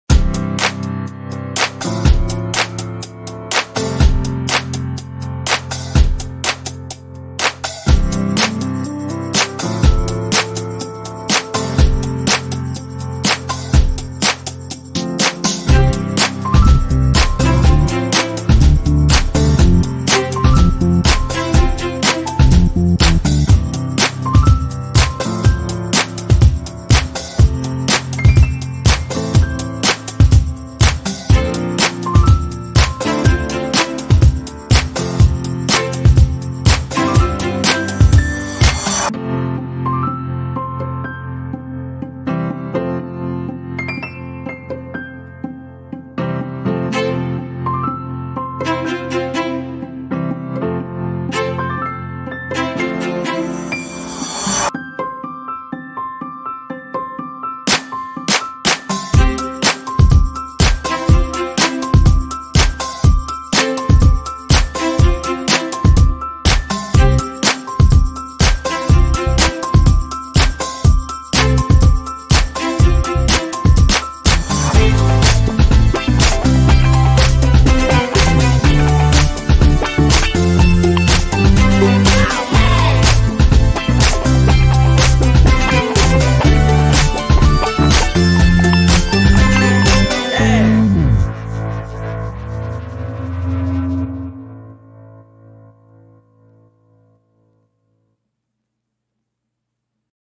iProductionManager. Complex productionrequires simple solutions. | RATIONAL
The iProductionManager plans production processes and indicates
which foods are best cooked together. Streamlines processes, saves
time and energy.

 Life in the kitchen: the stress and rush to get everything ready at the
same time and on time. A logistical challenge whether using pen and
paper or software. No more. Because now the iCombi Pro with the
iProductionManager are taking over: Simply place the dish on the display and it will show you what else you can produce at the same time. You
only have to specify whether you want to cook with time, or energy
efficiency. The system will monitor each rack individually, so that the
cooking times are adjusted intelligently to the quantity and desired result.
You decide if you want the food to start cooking at the same time, or
finish cooking at the same time. Either way, the iCombi Pro will tell you when something needs to go into the cooking cabinet - et voilà, the food is ready.

► Experience RATIONAL on other Social Media Channels